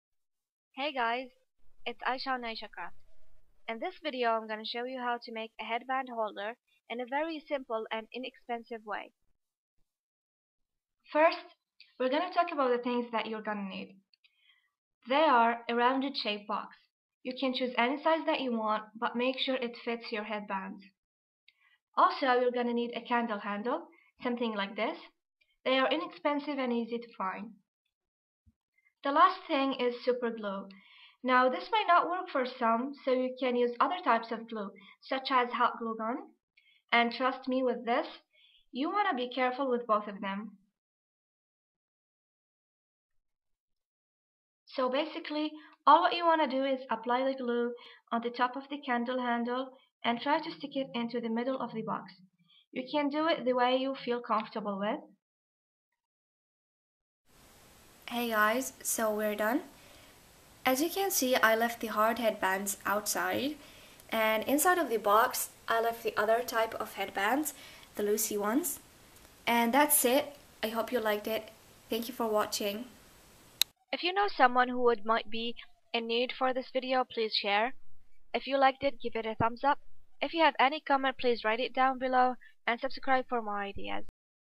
DIY: Ultimate headband holder Simple easy and inexpensive
Things you will need :
1. a rounded shape box (size is optional )
2. candle handle
3. superglue ( or any type that will work )